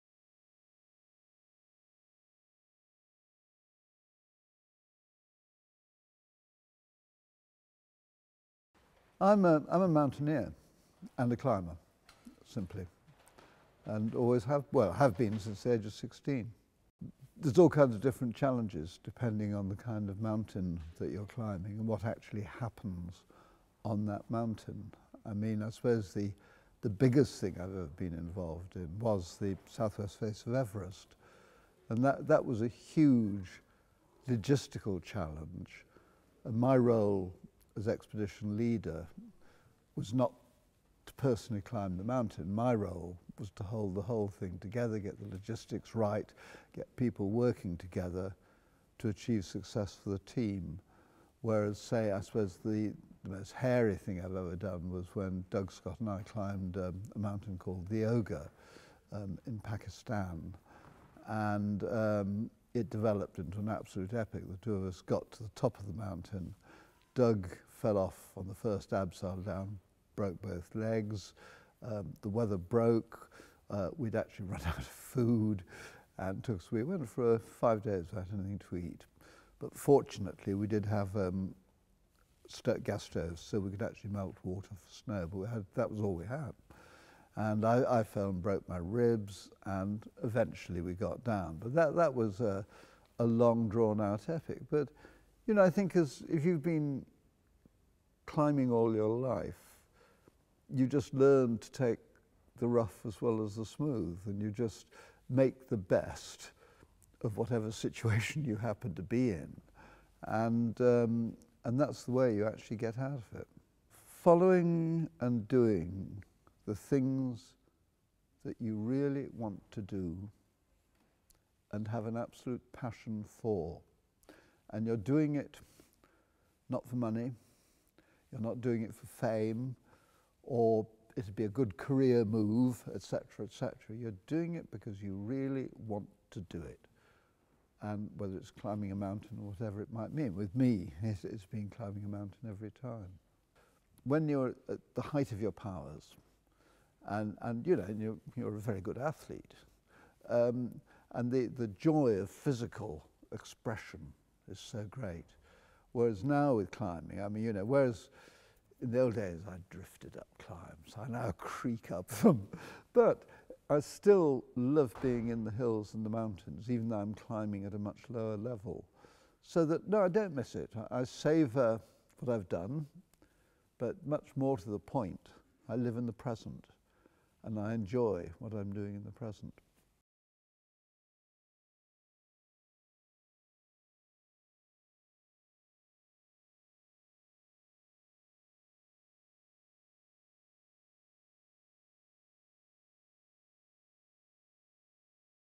An Interview with legendary climber Sir Chris Bonington
A rare interview with Sir Chris Bonington about life, surviving incredible hardship and climbing Everest. Features footage from "The Himalayan - Mt.Everest Base Camp trek HD Time Lapse" by Amit Aware. Filmed to support the Ice Warrior Project — with David Hewitt, Ralph Cochrane and Jim McNeill at Royal Geographical Society.